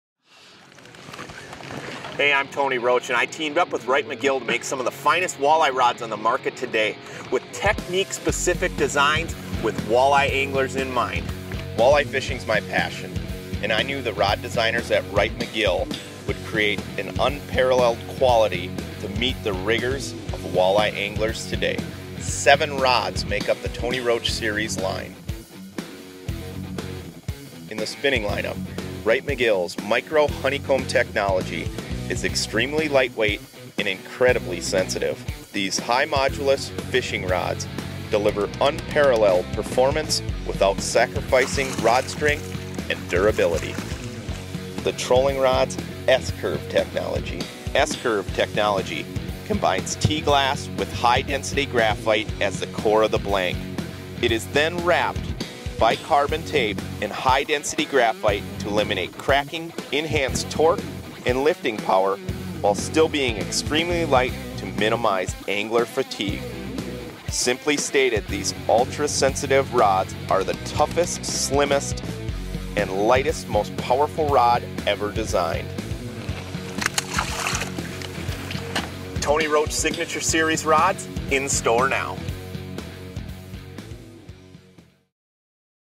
Tony Roach Signature Series Rods by Wright & McGill
New for 2013, 7 rods in a variety of actions, make up the Tony Roach Series. Designed by a Walleye angler, for the Walleye angler. Get yours where ever rods are sold!